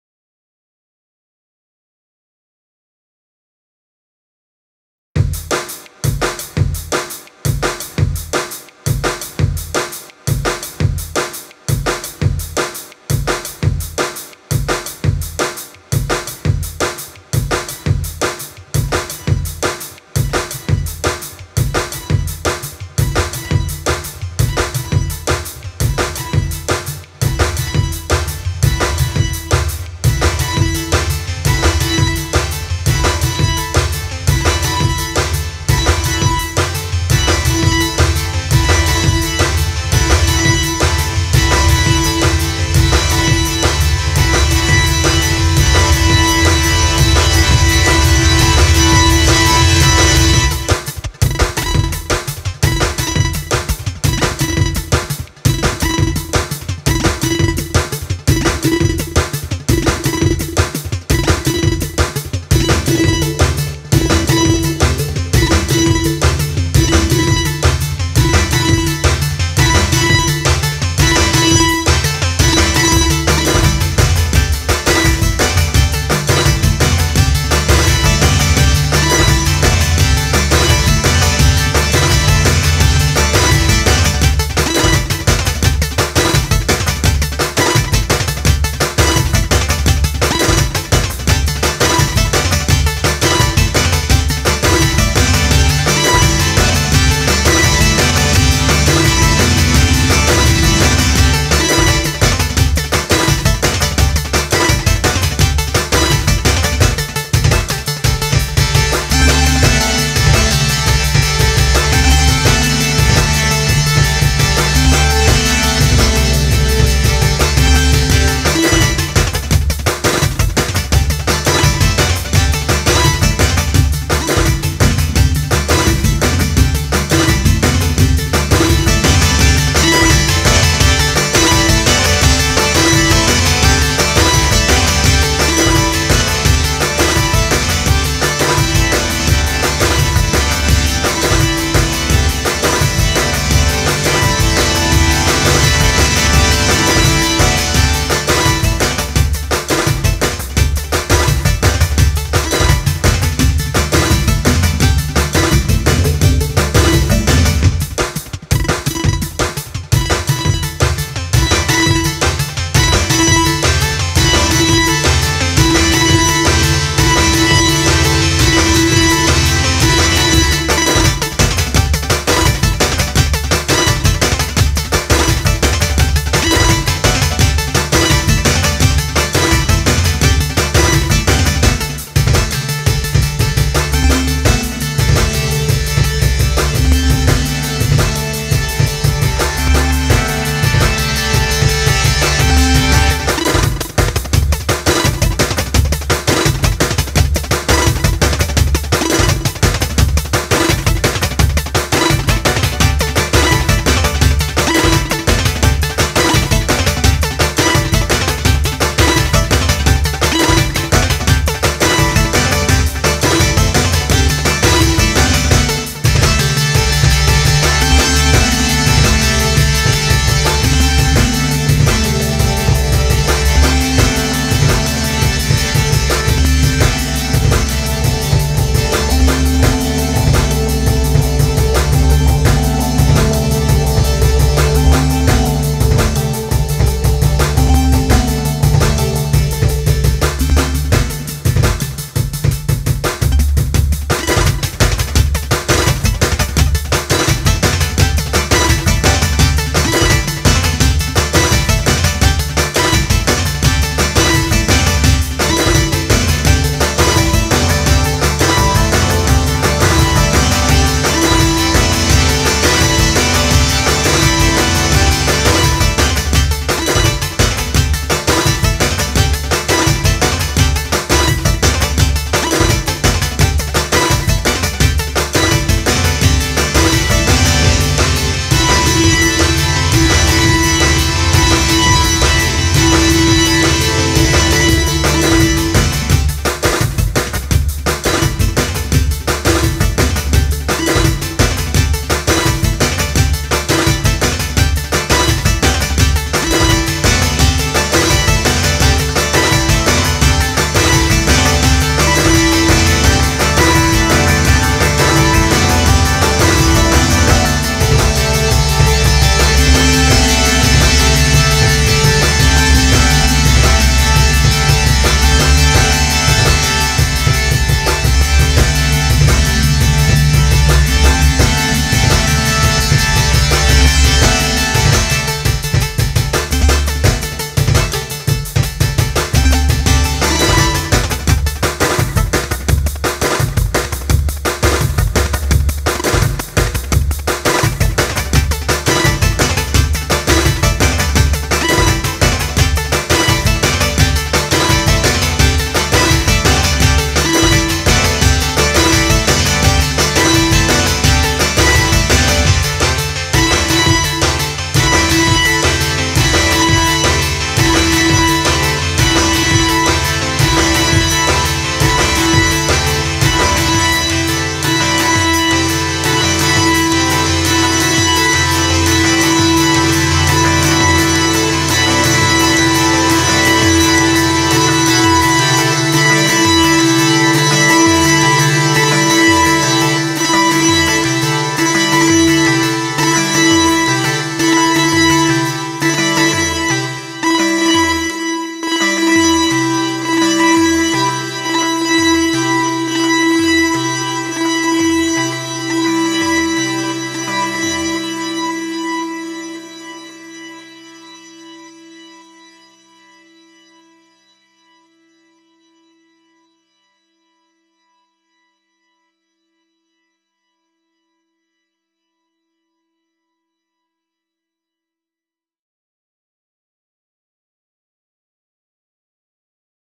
Machinedrum MKII UW+ & Virus Ti - First Jam
Hi all to those that bother reading the descriptions :@)

this is the first real play I've managed to have with both these bits of kit and I have to say it was a true pleasure its a tad ropey here and there but I wanted to capture it as live as I could and just have some fun , that said I hope you like it despite the flaws , both these bits of gear are just so intuitive to play around with live once you get over the odd hurdle , and read a manual or two and some forums and more manuals... ( you get the picture ) but definitely well worth the effort and I can't wait to continue to grow with them.
much love to all of you out there in virtual land .. here's some links if you fancy following my work , also I may finish this idea and give it away on soundcloud so keep those eyes peeled if you like it..

Peace . L